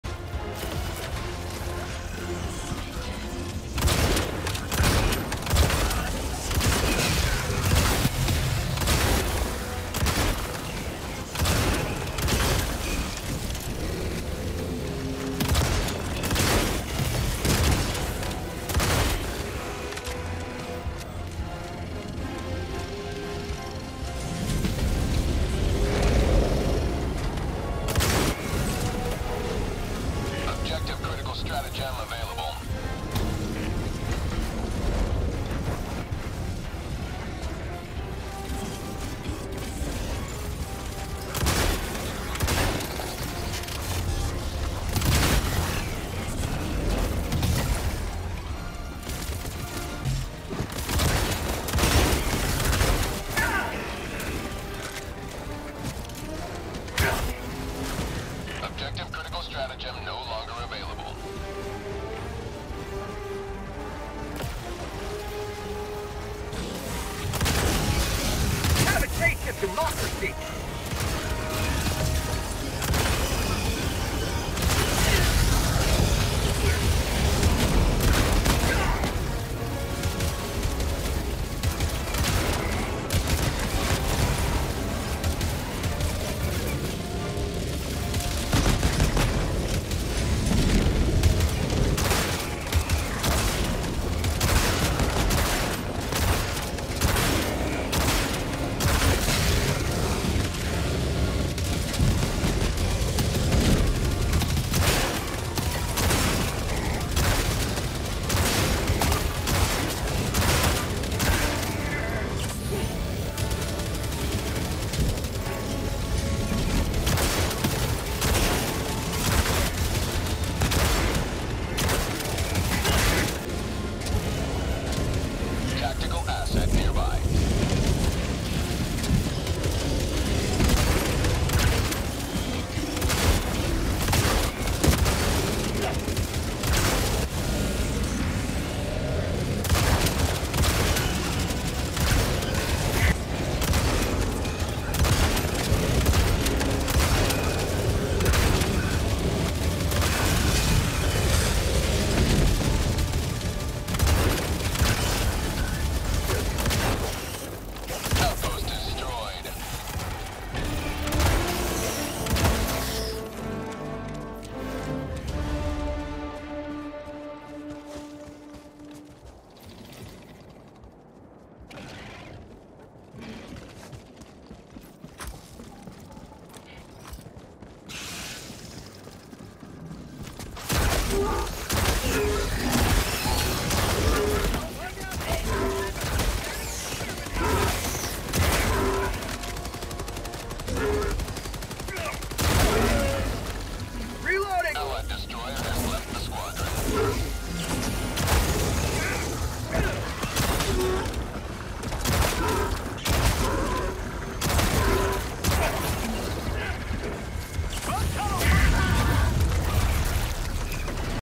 Yes… I’ve Found My New Favorite Shotgun
Testing out new HG-20 Shotgun from Truth Enforcers Warbond